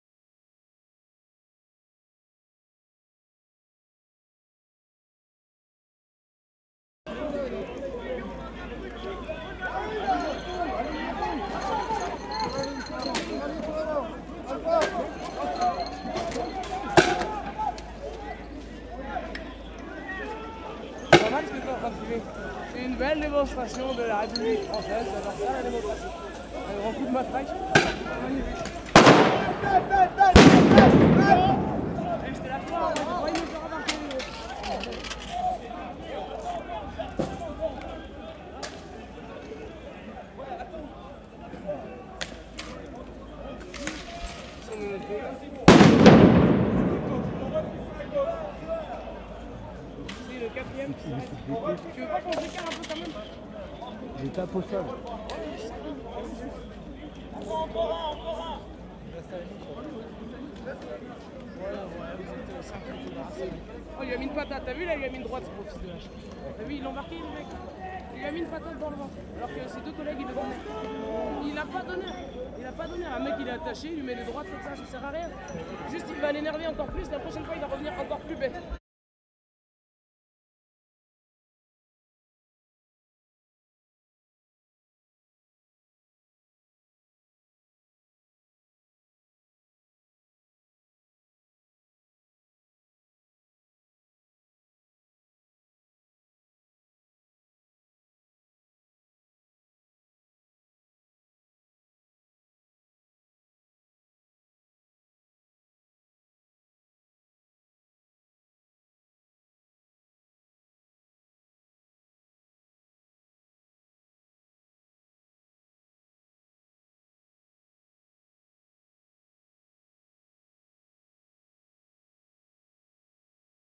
Des manifestants de Nuit debout menottés frappés par des CRS
Dans la nuit du jeudi 28 au vendredi 29 avril 2016 place de la République à Paris, lors de l'évacuation très musclée de la Nuit debout, des coups assénés par des policiers ont été donnés sur le visage de plusieurs manifestants, alors que ceux-ci ne montrent aucun signe de résistance.